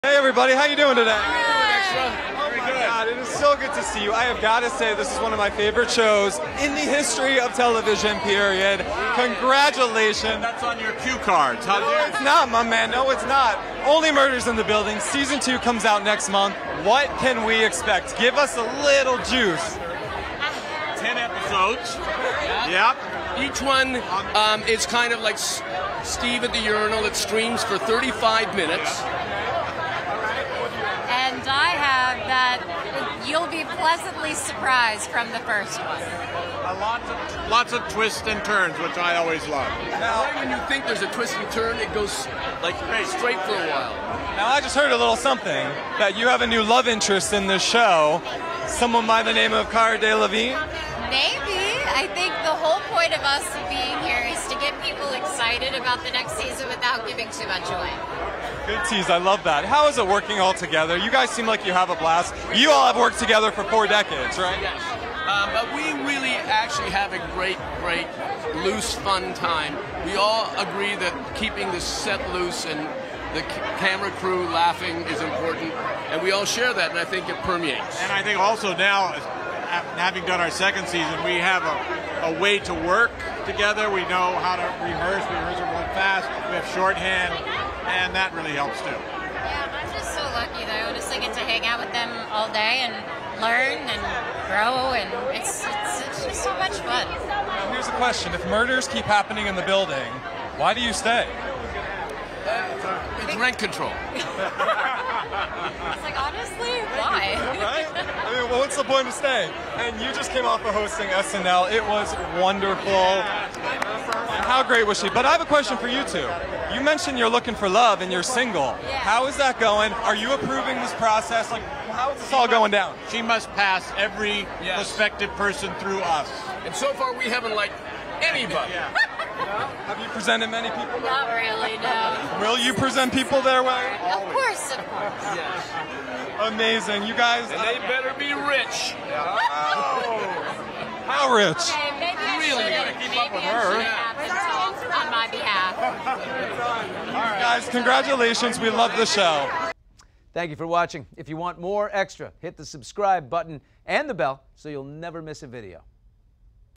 Selena Gomez on New ‘Only Murders’ LOVE Interest and Her Own Dating Life (Exclusive)
Selena Gomez, Steve Martin and Martin Short are back for Season 2 of ‘Only Murders in the Building,’ and they teased what to expect in a new interview with ‘Extra’s’ Tommy DiDario at ABC Upfronts! Selena played coy about the buzz that Cara Delevingne is playing her love interest in the new season. Plus, after Selena revealed on ‘SNL’ that she’s single and looking, Martin and Steve say they’ll have to give the seal of approval! ‘Only Murders in the Building’ is back on Hulu June 28.